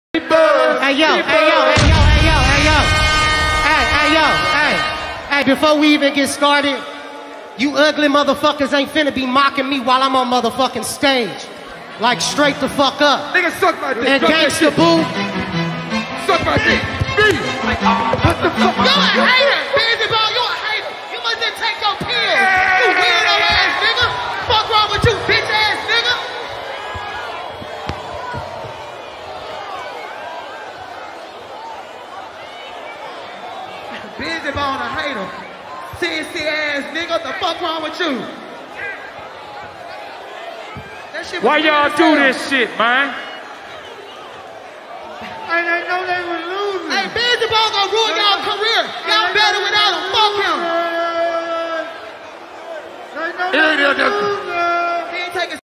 Bizzy Bone presses 36 Mafia OVER MOCKING VERZUZ Bone Thugs N Harmony vs Three 6 Mafia Fight! 1080P
bizzy bone verzuz three 6 mafia on spotify